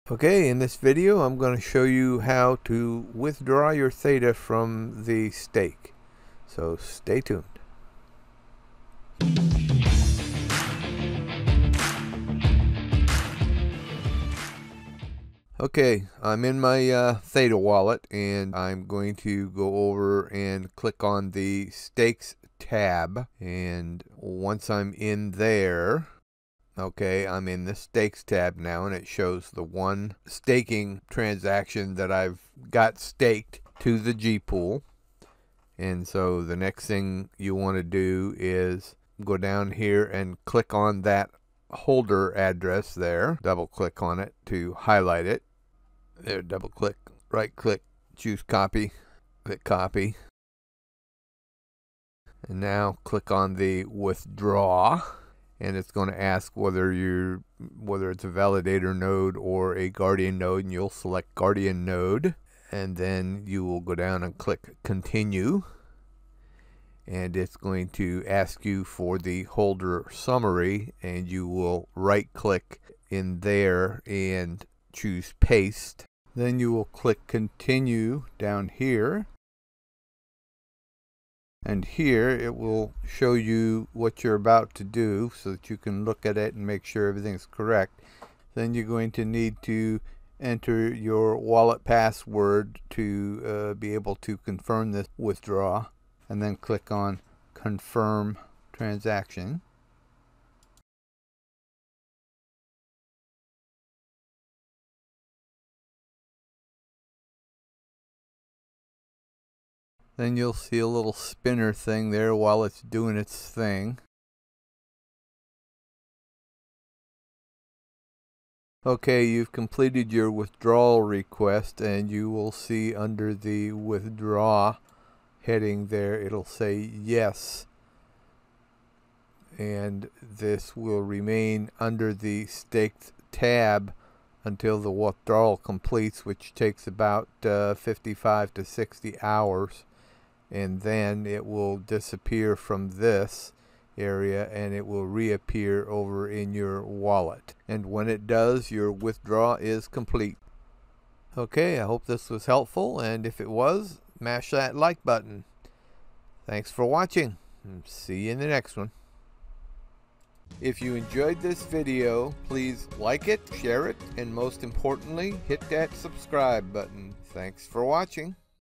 How to withdraw your staked theta
How to withdraw your staked theta:  In this video I show you step by step how to withdraw your staked theta.